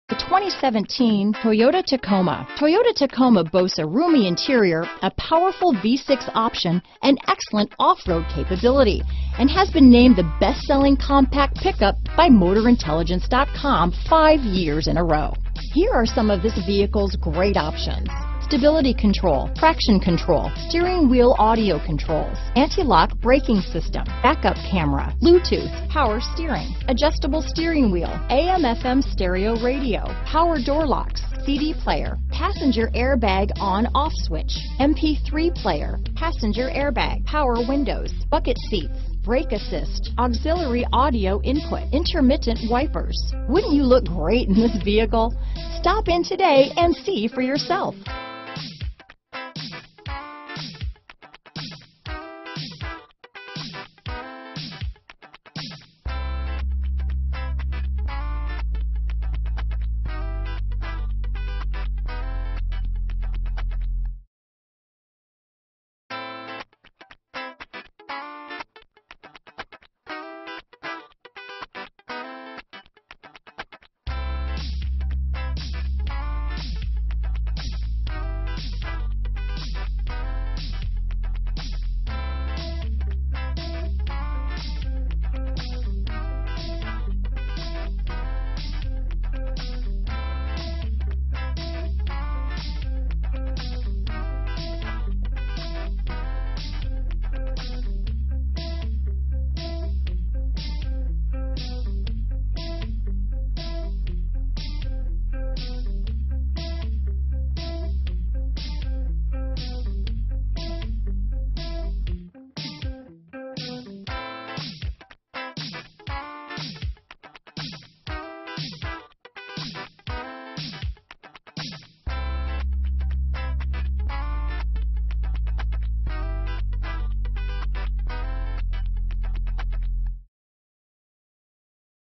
2017 Toyota Tacoma Longwood, Orlando, Lake Mary, Sanford, Miami, FL KHX079955
Parks Toyota
1701 South Woodland Boulevard

The Toyota Tacoma delivers unwavering capability and the style to match. Chiseled lines and muscular fenders help show off Tacoma’s strength, and the available hood scoop with aggressive grille tease at the power that hides under the hood. Its interior offers soft-touch materials to boost your comfort. Tacoma Limited adds new leather-trimmed seating surfaces for the ultimate in tough sophistication. An available 4.2-inch color Multi-Information Display helps keep an eye on the information you want. Customize it to show mpg, song selection, individual tire pressure, and more. When backing up you need to see what’s behind you. The standard backup camera mounted in Tacoma’s tailgate helps you see an image of what is behind you on the available 7-inch high-resolution display. New high-strength steel is used in the construction of every Tacoma, helping ensure its toughness. Stronger and lighter than conventional steel, this hot-stamped high-strength alloy optimizes safety and performance. Ready to hear her roar? Tacoma’s available Atkinson-cycle 3.5L V6 with direct injection is all-new and gives you the muscle you want. It’s got more power with 278HP and 265 lb.-ft. of torque at 4600rpm. The powerful 3.5L V6 and available Tow Package help the Tacoma tow up to 6,800 lbs. or carry a payload of up to 1,440 lbs. Tacoma’s TRD Off-Road’s Crawl Control is here to help you conquer the off road. This advanced system automatically modulates the throttle and brakes on five low-speed settings. Every Tacoma comes standard with our Star Safety System, an advanced integration of safety technologies including Vehicle Stability Control, Traction Control, Anti-lock Brake System, Electronic Brake-force Distribution, Brake Assist, and Smart Stop Technology.